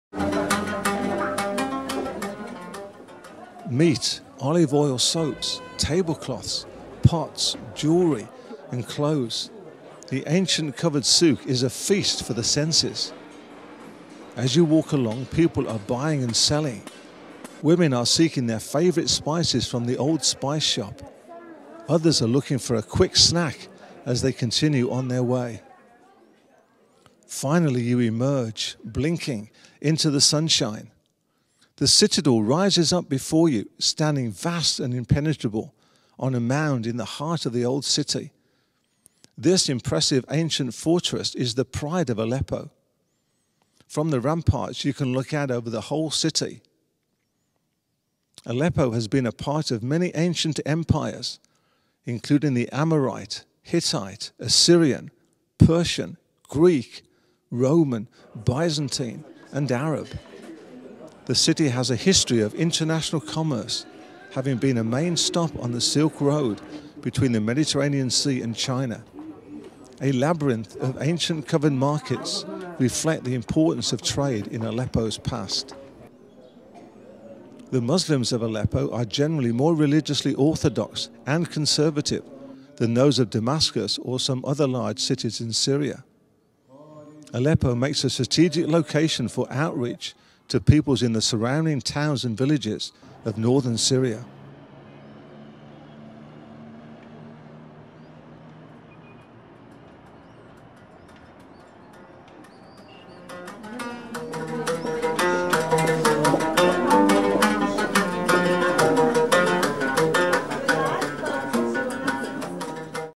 Aleppo, Syria
Calling Christians to prayer for the city of Aleppo Syria.